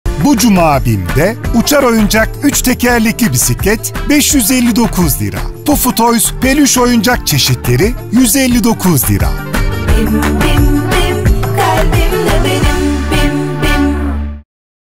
5 Temmuz | Oyuncak | BİM
Bu Cuma BİM'de Uçar Oyuncak Bisiklet 559,00 TL, Pelüş Oyuncak Çeşitleri 159,00 TL.